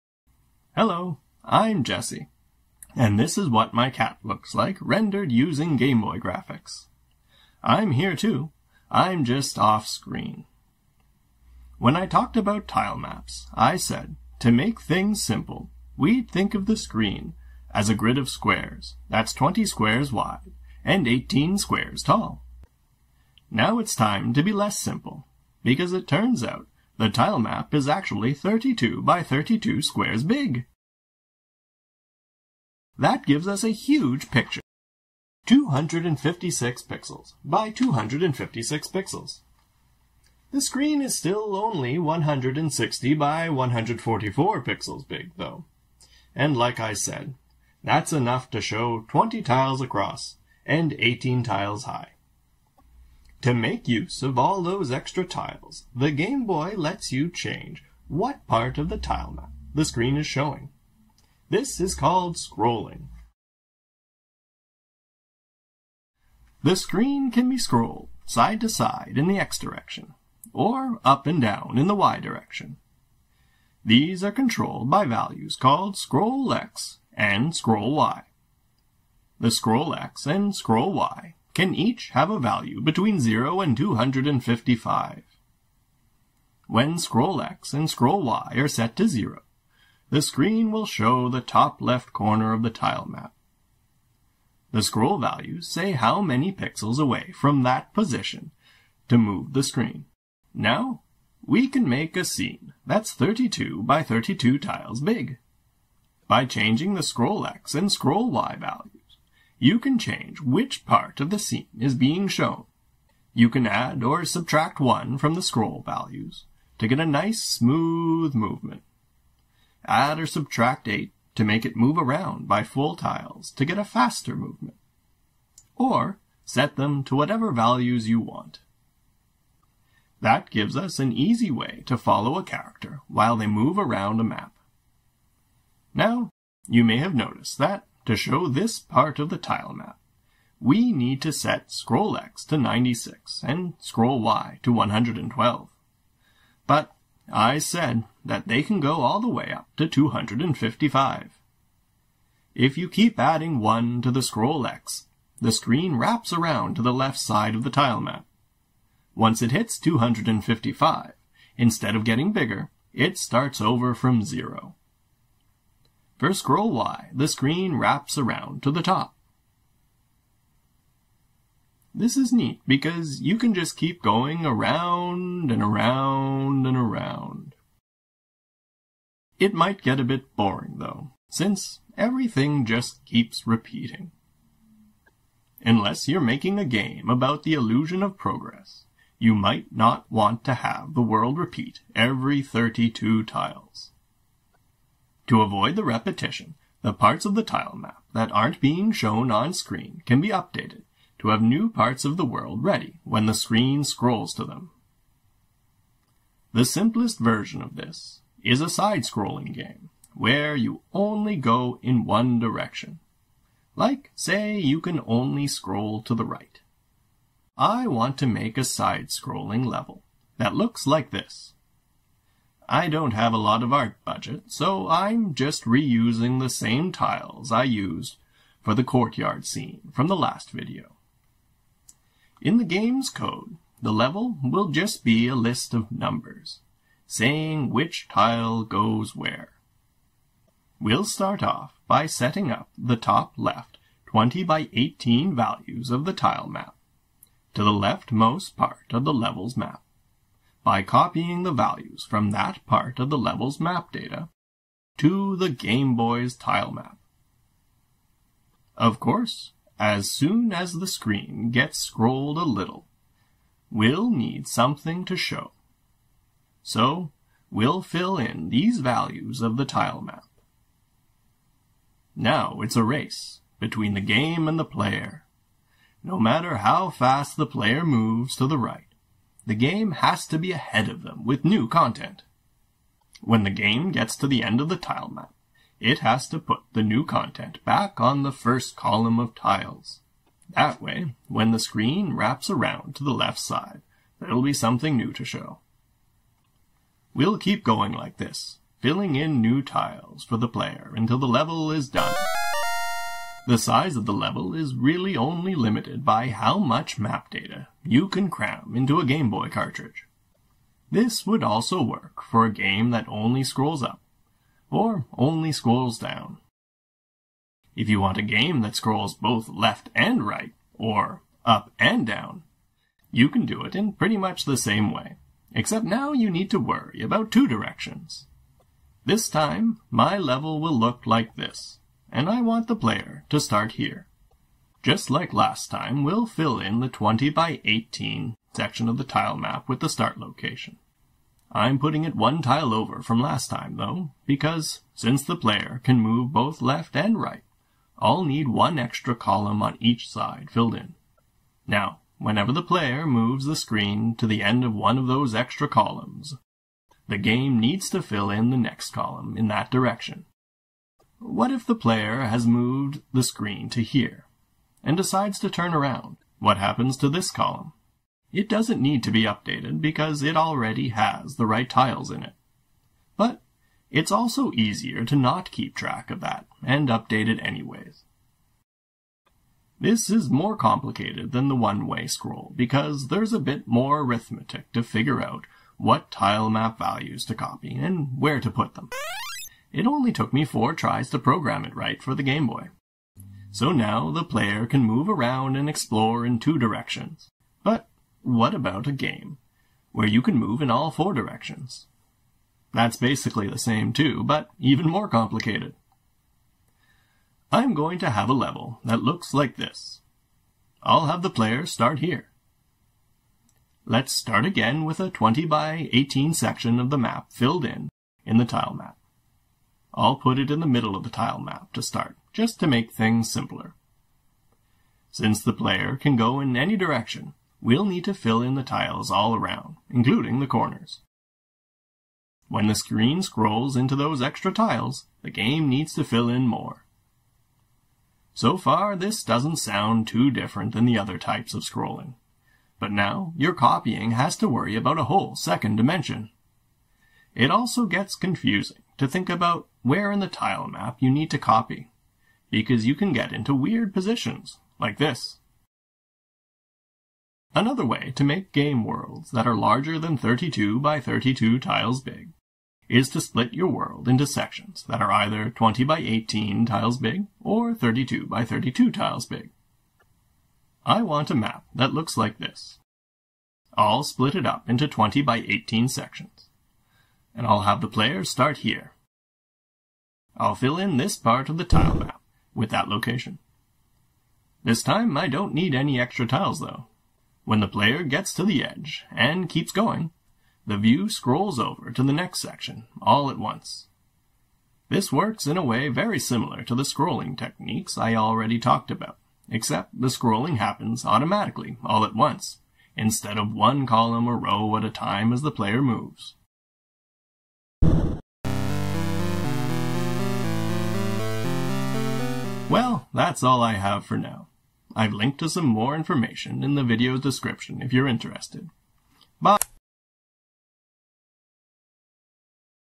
Background Scrolling - How GameBoy Graphics Work Part 3
Part 3 of a series explaining the basics of GameBoy graphics. This video covers scrolling.

If you want to know how I implemented a very large 4-direction scrolling "game" you can check out

For technical details about scrolling on the GameBoy, a good place to start is

If you want to see some example GameBoy assembly code for scrolling I have three demos implemented:

Video Chapters
0:00 Power on
0:14 Introduction
1:19 What is scrolling
2:29 Looping
3:40 Side scrolling games
5:31 Two direction side scrollers
6:51 Four direction scrolling
7:51 Room based scrolling
8:46 Conclusion

The GameBoy style video of me was processed using ffmpeg and Python.
The intro music and sound effects were made but it's played as an mp3.
The video was produced with OBS for screen capture, kdenlive for video editing, and audacity for audio editing.